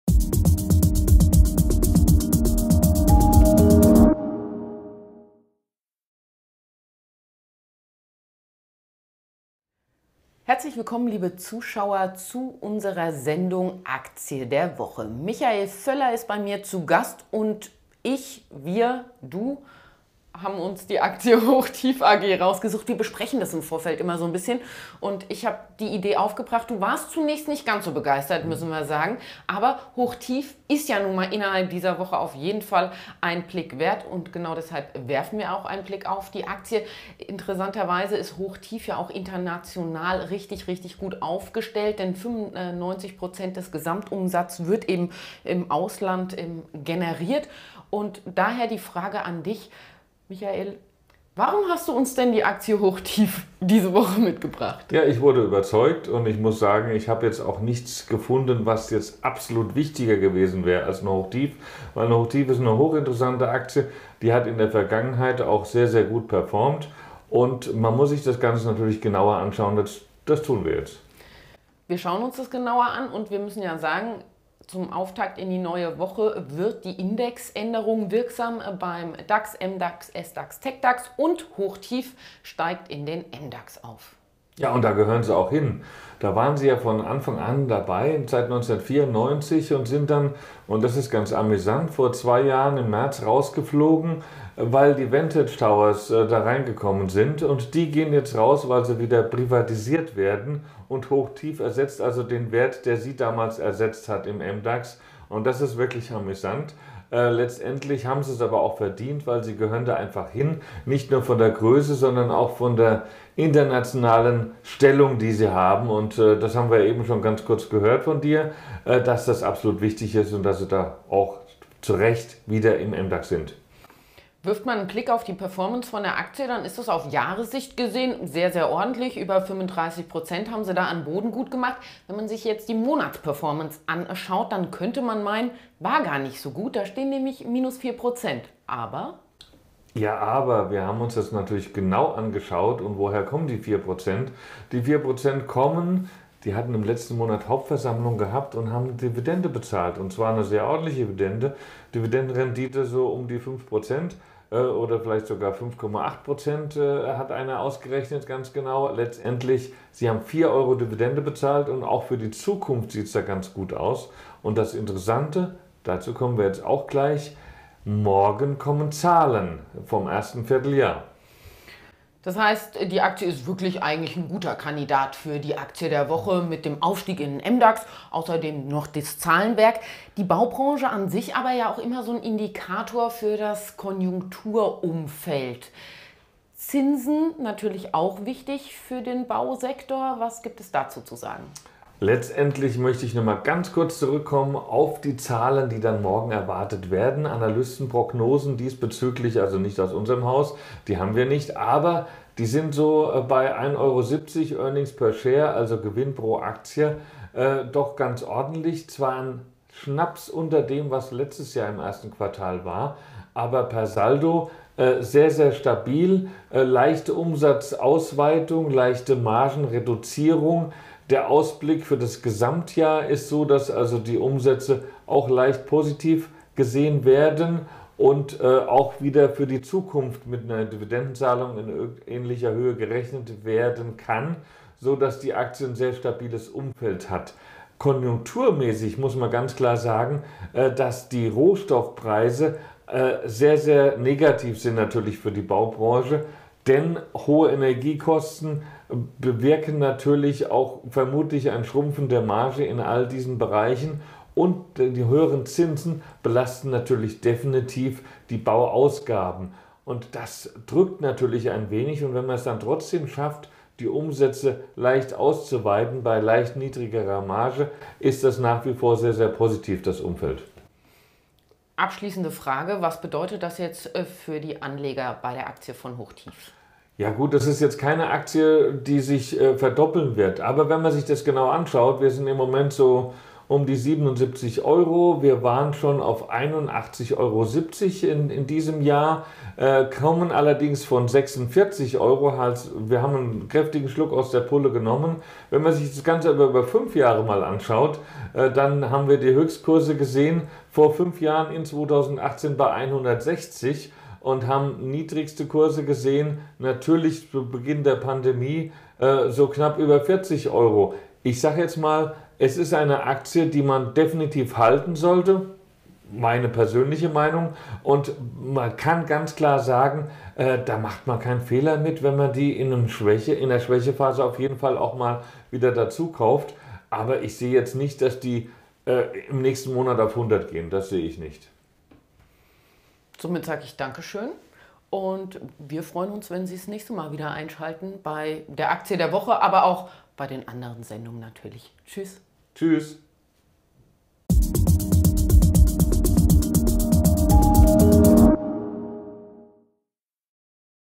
Aktie der Woche - HOCHTIEF AG
Hochtief AG ist die Aktie der Woche.
Michael Foeller, Director Global Markets der ICF BANK AG, hat sich den Baukonzern genauer angeschaut. Die Rückkehr in den MDax, aber auch die anstehenden Quartalszahlen stehen im Fokus.